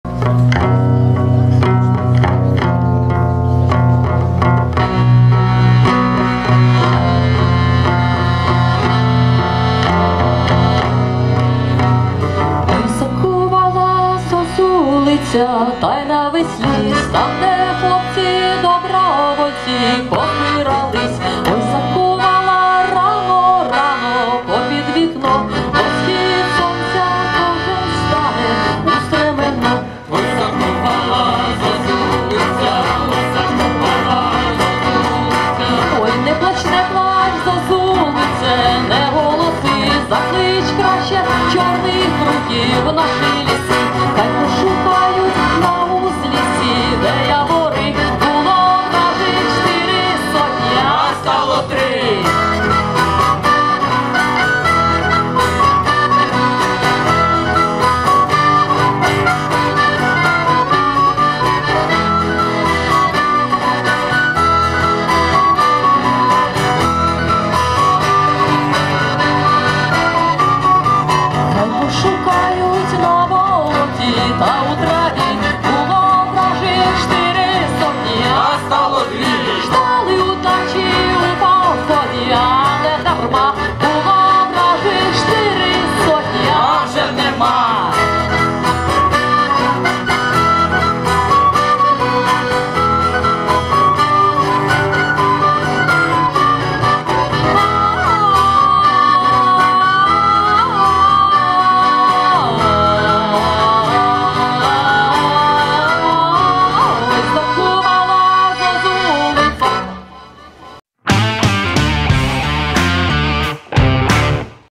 Гурт Телері 5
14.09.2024 відбувся 7-мий Літературно-музичний фестиваль «Нескорений ПроRock» у Cквері імені Василя Сліпака за підтримки ПоділНескорений.